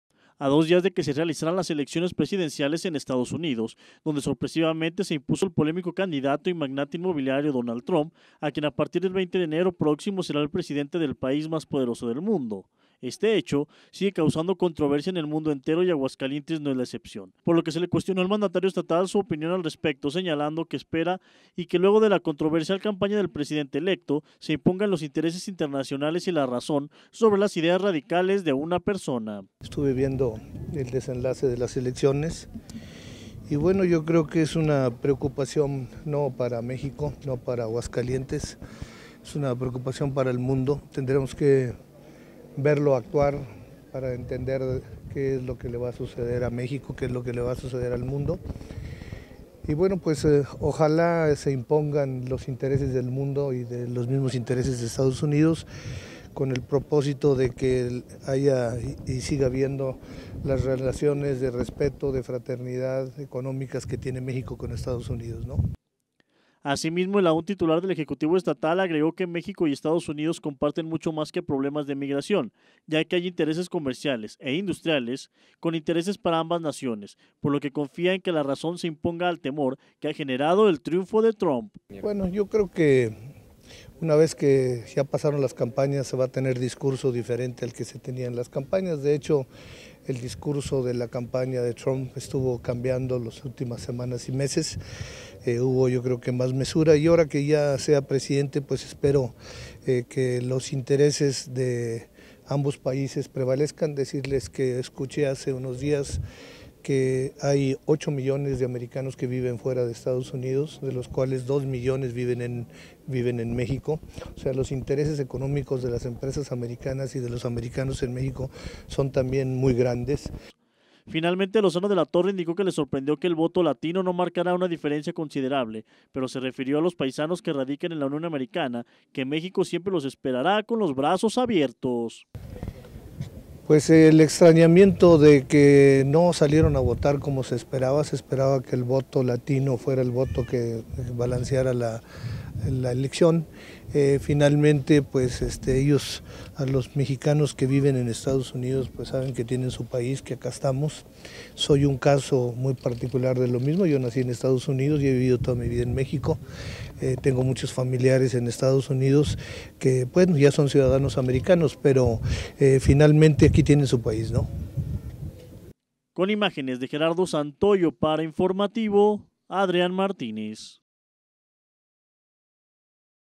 TRUMP - LOZANO
Se debe imponer los intereses del mundo y no los de una persona, asegura el Gobernador de Aguascalientes, Carlos Lozano de la Torre, ante el triunfo de Donal Trump.